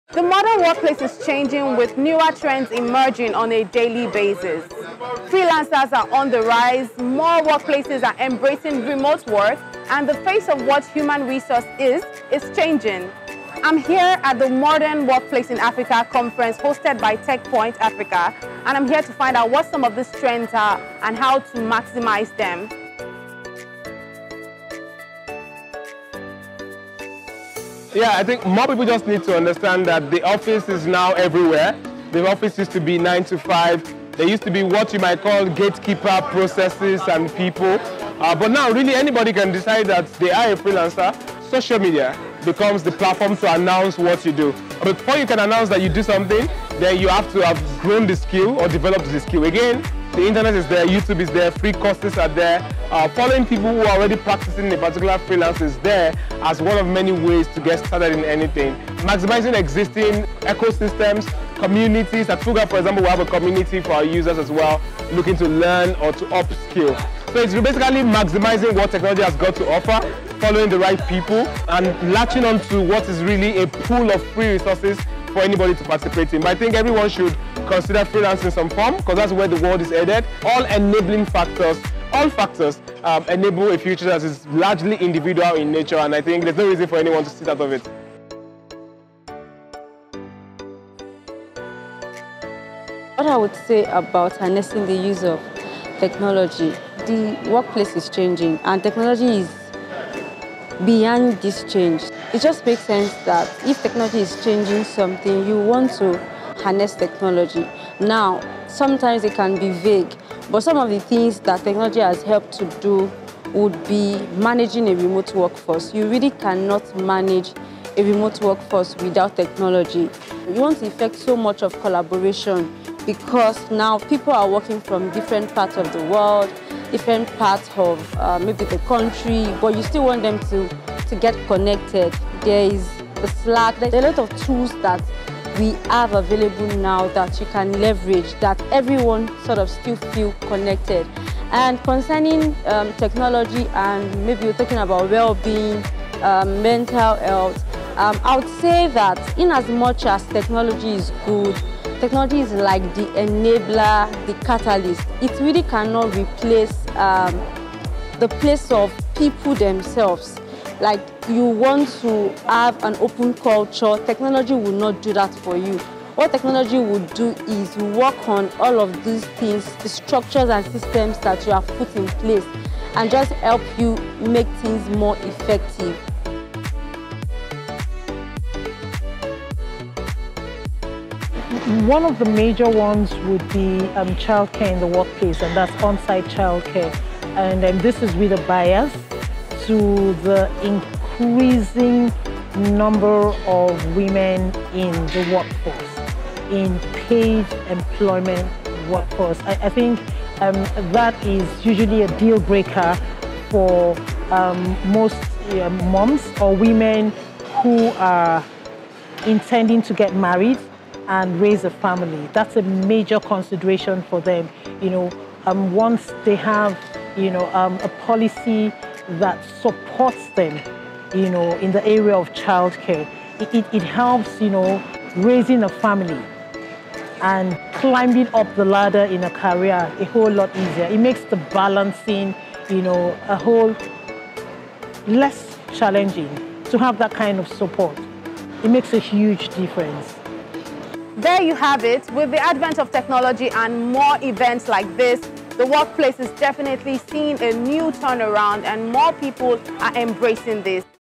Newer trends to look out for in the modern workplace
The modern modern workplace goes beyond technology, it’s about embracing a culture that fits today’s environment.

Here are some insights from @techpointafrica’s “Modern Workplace Africa” conference